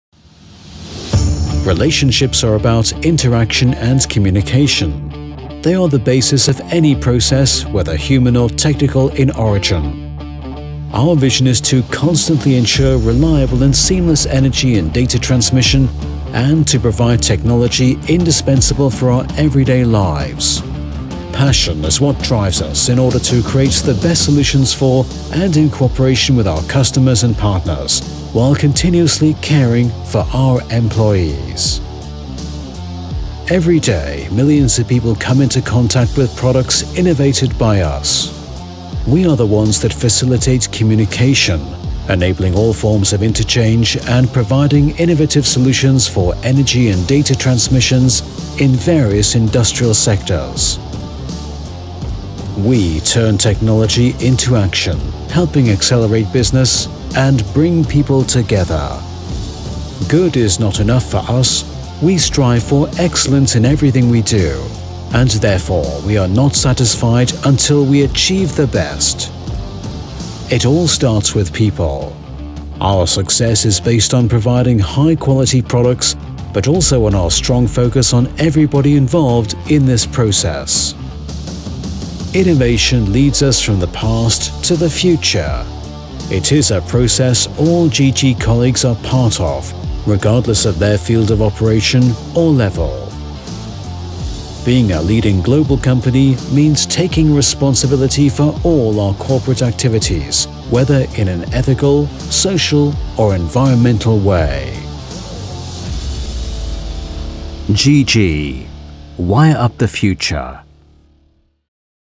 GG Group | Corporate Video
GG Group leads through pas­sion in en­ergy & data trans­mis­sion - We are a global, fam­ily-owned busi­ness group, producing tech­ni­cally ad­vanced high-qual­ity wires and har­nesses for au­to­mo­tive and in­dus­trial ap­pli­ca­tions. Find out more about GG in our corporate video.